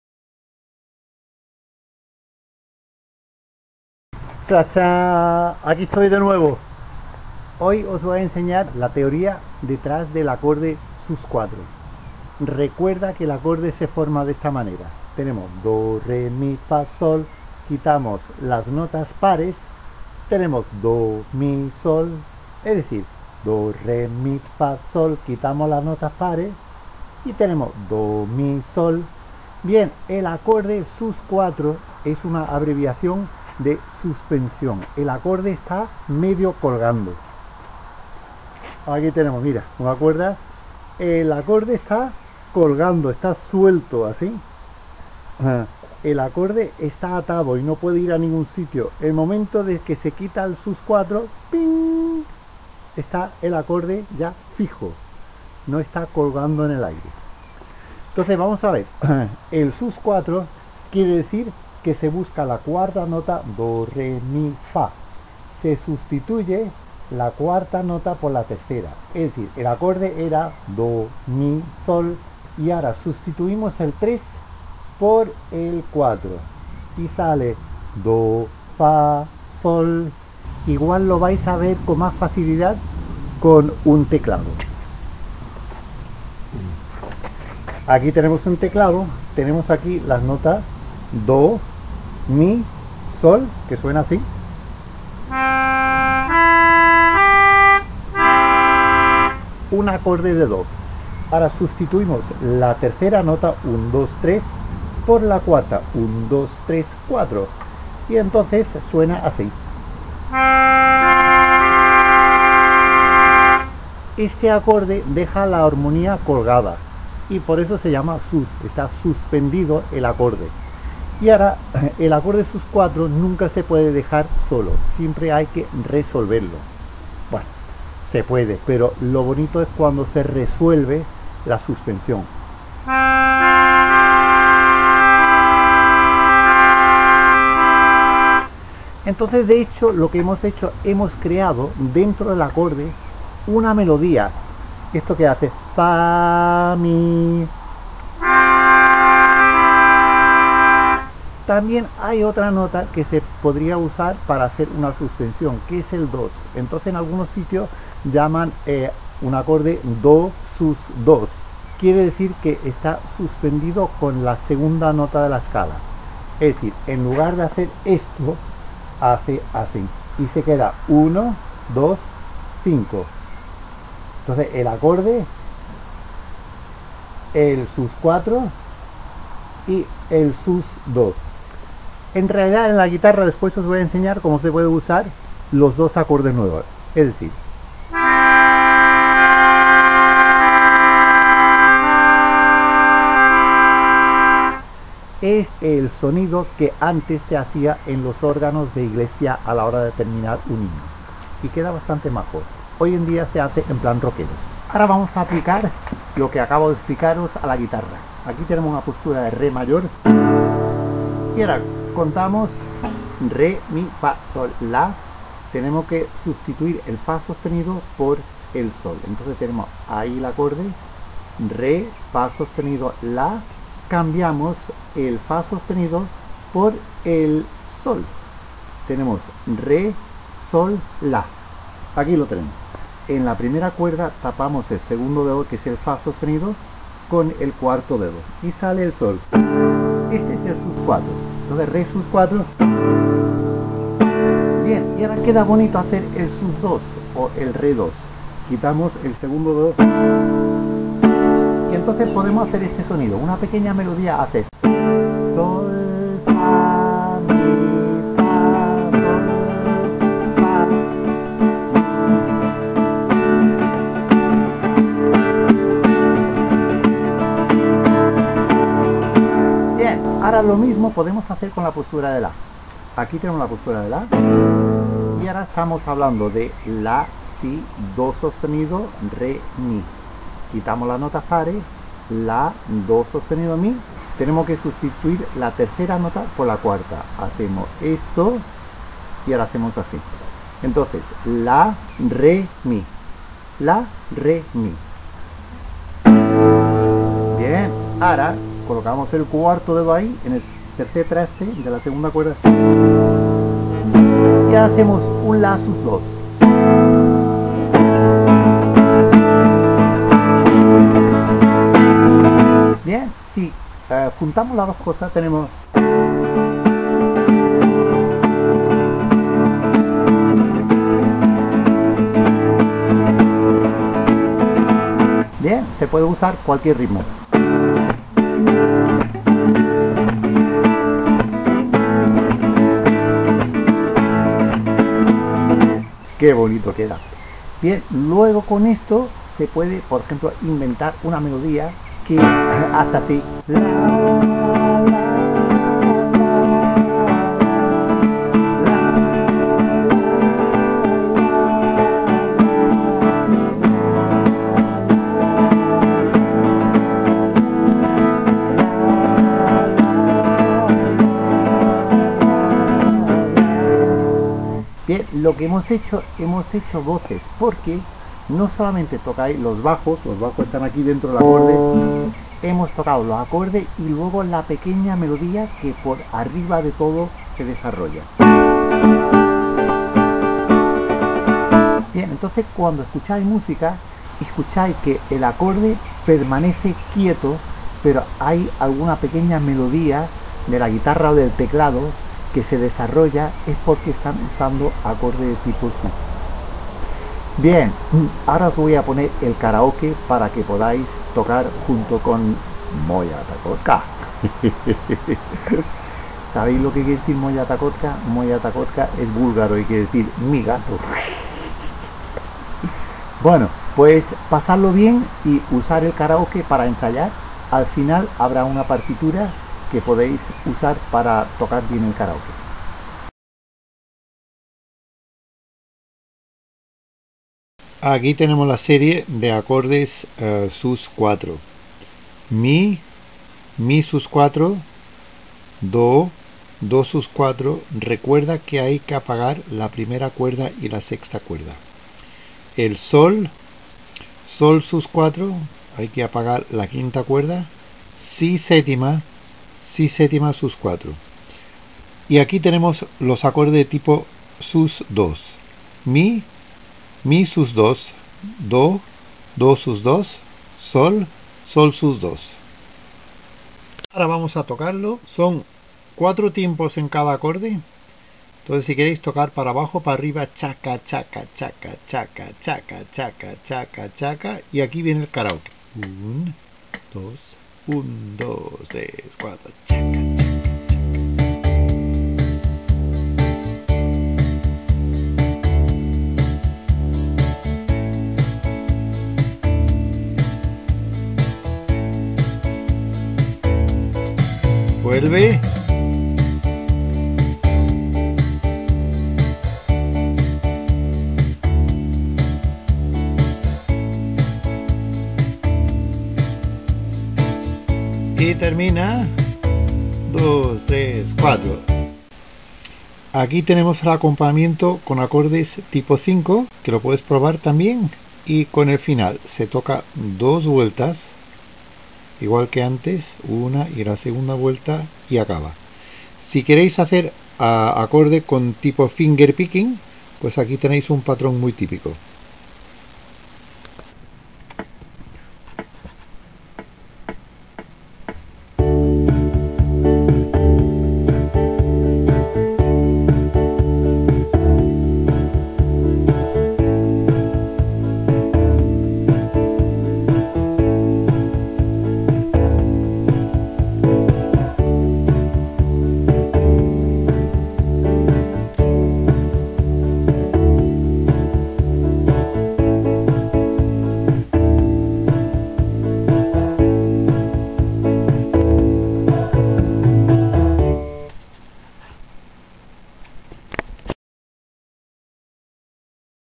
Aprende el acorde sus4
Clase particular con explicación, demostración, lista de acordes, partitura y "jam-track" (karaoke).